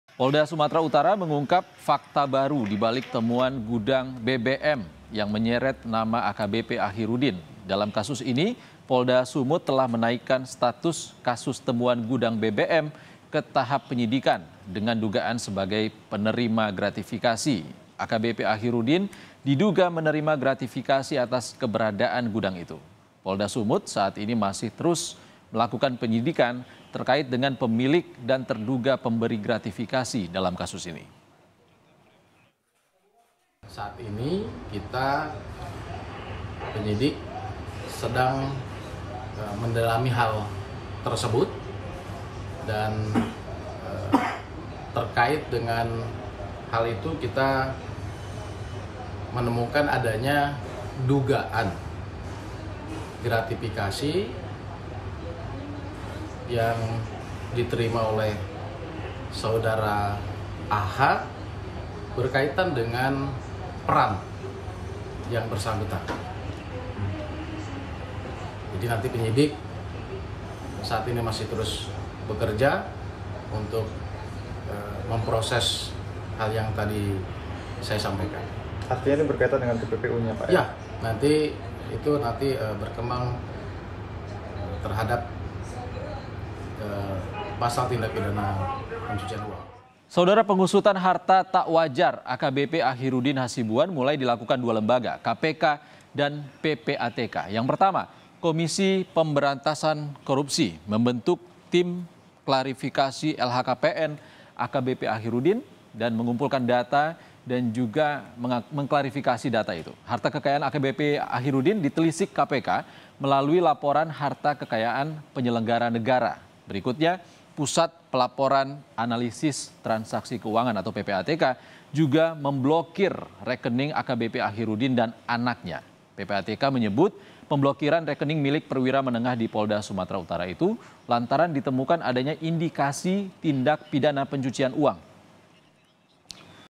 Membongkar Fakta Baru AKBP Achiruddin Soal Gudang BBM dan Harta Tak Wajar, Siapa Pemasoknya?
MEDAN, KOMPAS.TV - Polda Sumatera Utara, mengungkap fakta baru dibalik temuan gudang BBM yang menyeret nama AKBP Achiruddin.

Dalam kasus ini, Polda Sumut telah menaikkan status kasus temuan gudang BBM ke tahap penyidikan dengan dugaan sebagai penerima gratifikasi.

AKBP Achiruddin, diduga menerima gratifikasi atas keberadaan gudang itu.

Polda Sumut, saat ini masih terus melakukan penyidikan, terkait dengan pemilik dan terduga pemberi gratifikasi dalam kasus ini.

Pengusutan harta tak wajar AKBP Achiruddin hasibuan mulai dilakukan KPK dan PPATK.

Yang pertama, Komisi Pemberantasan Korupsi bentuk tim kumpulkan data dan klarifikasi harta AKBP Achiruddin.

Harta kekayaan AKBP Achiruddin ditelisik KPK melalui Laporan Harta Kekayaan Penyelenggara Negara.

Berikutnya, Pusat Pelaporan dan Analisis Transaksi Keuangan (PPATK) juga memblokir rekening AKBP Achiruddin dan anaknya.

PPATK menyebut pemblokiran rekening milik perwira menengah di Polda Sumatera Utara itu, lantaran ditemukan adanya indikasi tindak pidana pencucian uang.